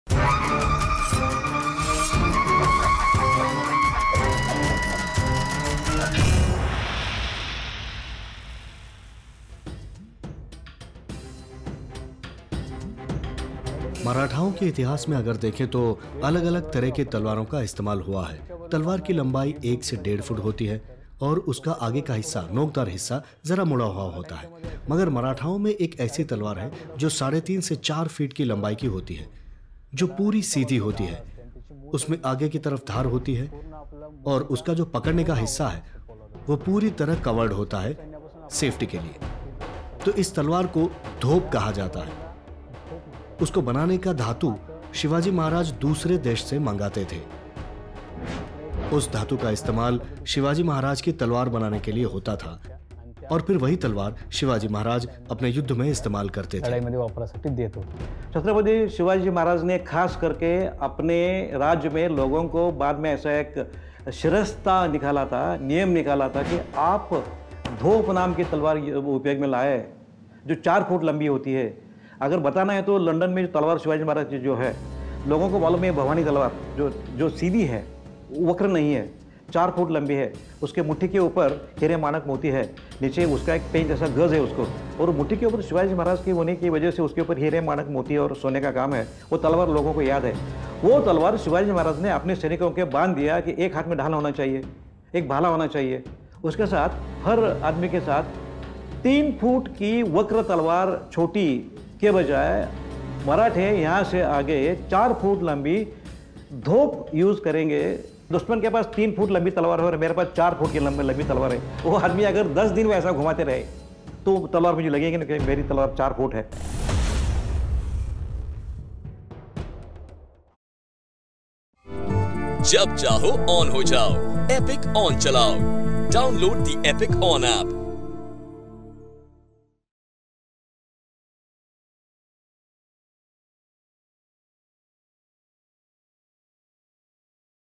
शस्त्र का शास्त्र | Indian Martial Arts- Dhop
A weapon-based Indian martial art, Mardani Khel originated in Maharashtra among the Marathas in the early 1600s. The Marathas were homespun warriors whose martial art was distinctive because of the use of the Pata (sword) and the Vita (corded lance). Mardani Khel rose to prominence under the leadership of Chhatrapati Shivaji Maharaj, who relied heavily on the guerrilla tactics of Maratha units. Today, Mardani Khel exists only as a performing art and as a powerful reminder of the Maratha valiance.